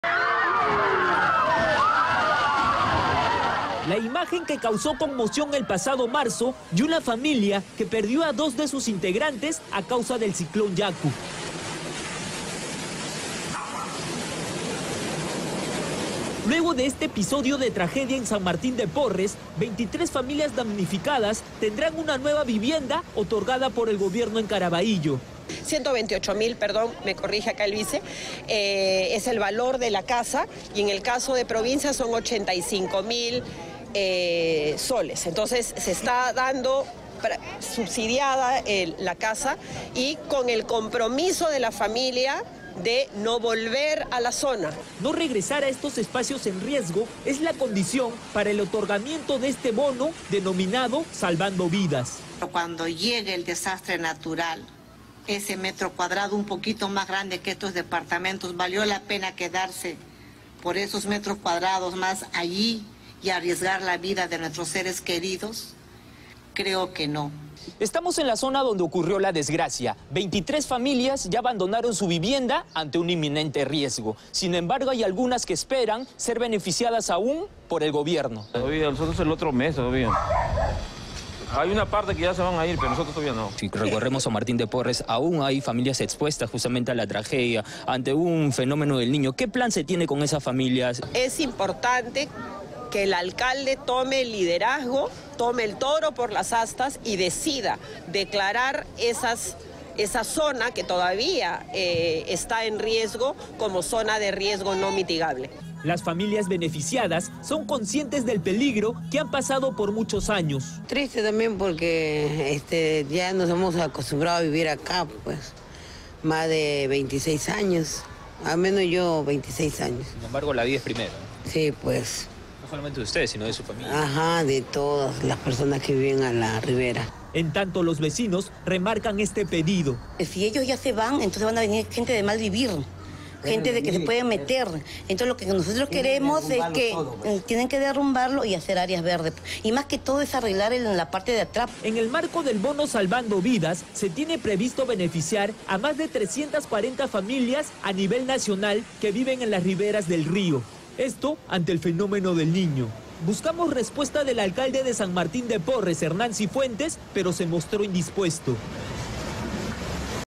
Gobierno entrega casas seguras a familias damnificadas a causa del ciclón Yaku en Lima
El gobierno, a través del Ministerio de Vivienda, Construcción y Saneamiento, entregó casas seguras, financiadas con el bono "Salvando Vidas" a las primeras 23 familias damnificadas a causa del ciclón Yaku en Lima.

El acto de entrega fue encabezado por la presidenta, Dina Boluarte, quien estuvo acompañada de la ministra de Vivienda, Hania Pérez de Cuéllar, y se desarrolló en el distrito de Carabayllo.

En esta oportunidad, las autoridades entregaron simbólicamente las llaves de sus nuevas viviendas a 23 familias del distrito de San Martín de Porres (SMP), que perdieron sus viviendas a causa del ciclón Yaku.

BONO "SALVANDO VIDAS"

En el marco del bono "Salvando Vidas" se tiene previsto beneficiar a más 340 familias a nivel nacional que viven en las riberas del río, esto ante el Fenómeno de El Niño.